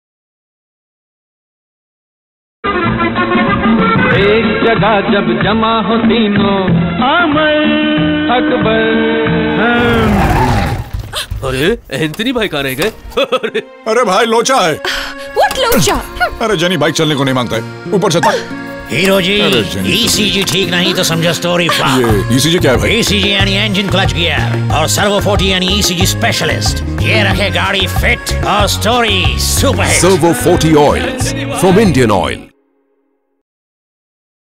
Servo - Amar Akbar Anthony.mp4
This commercial was shot for Servo 4T Oil.
Producer - Subrat Ray, Director - Gajraj Rao, Writer - Rohit Dubey,
DOP - V. Manikandan, Art Director - Navin Kenkre, Sound Engineer - Arun Crasto, Music Director - Amartya Rahut, Offline Editor - Kiran Ghaisas, Production House - code red films, Agency - Grey